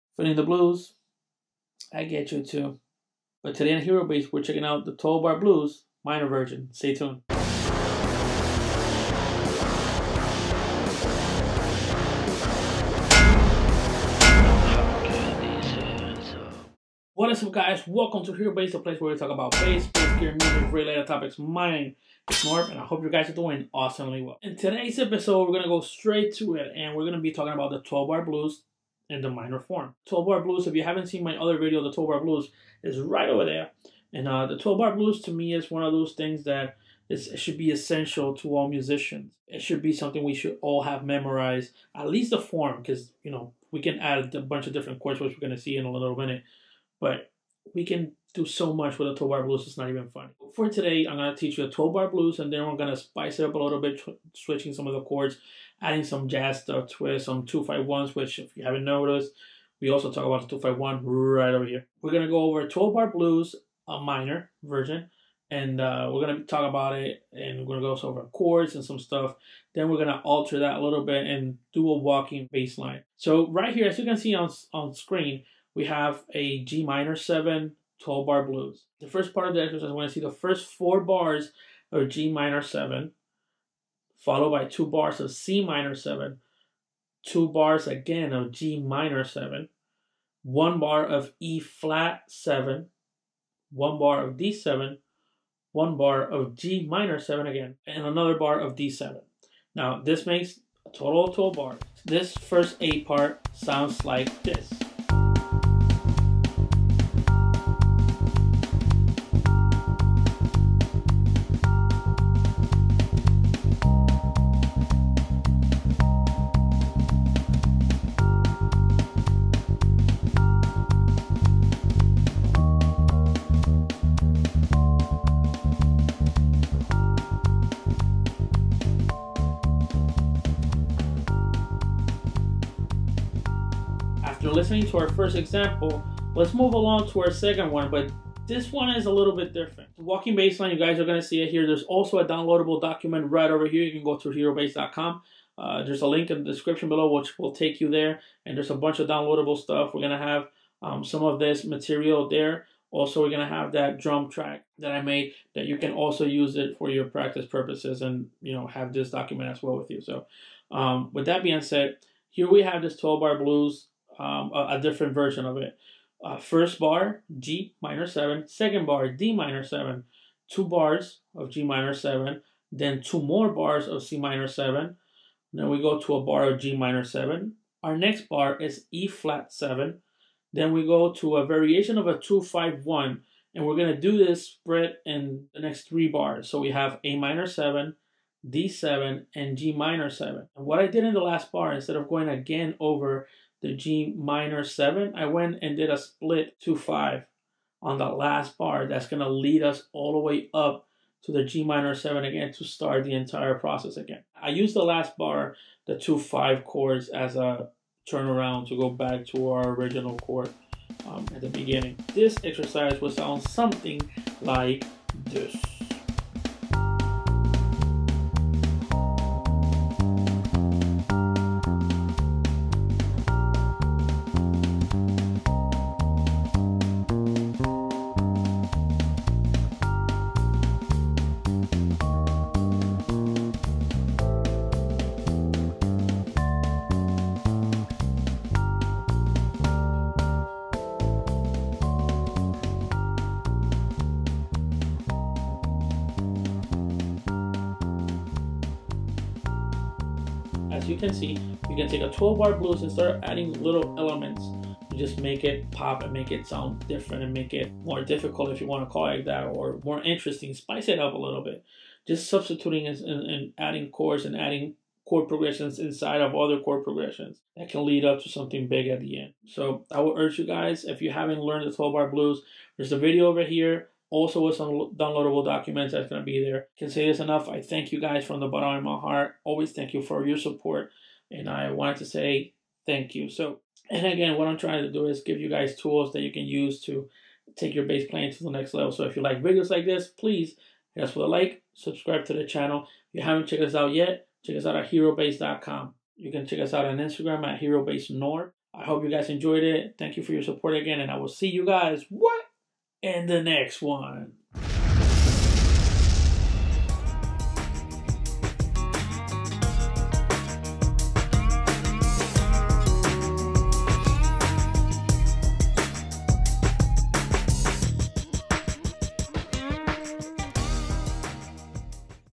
12 Bar Blues Minor
In today's EP:121, we will take a look at the 12 Bar Blues in a Minor version. We Will explain the format and the structure in the key of Gm. Hope this lessons helps and thanks for your continues support. We are always thankful.

- Focusrite 18i8 (V2)

- iPad (2020)

- Shure SE215 (IEM)

-ErnieBall Strings